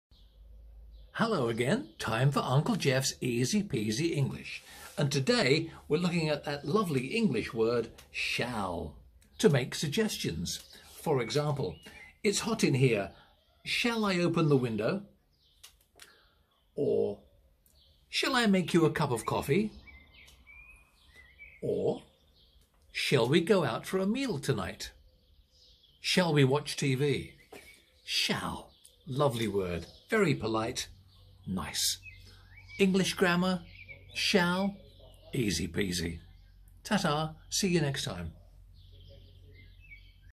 How to use SHALL in a sentence - English Grammar Lessons with Uncle Geoff
Learn English Grammar - How to use SHALL in a sentence. Also, what is the pronunciation of 'shall'?

In this lesson, you will learn how to improve your English grammar by using 'shall' in a sentence or question.

Shall is quite a formal-sounding word in English to refer to a future time...and makes you sound very polite when you use it. We use it to get agreement on an idea or suggestion, such as "It's hot in here. Shall I open the window?" or "I don't want to eat at home tonight. Shall we go to a restaurant?"

If you want to learn how to speak English fluently, you should learn how to use shall in a sentence or question...and Uncle Geoff's Easy-Peasy English grammar lessons will help you.

We often use 'shall' in asking questions, but it can also be used in a very profound way, such as, "When I am old, I shall wear purple' (from a poem called 'Warning' by Jenny Joseph).

English grammar...shall...easy-peasy!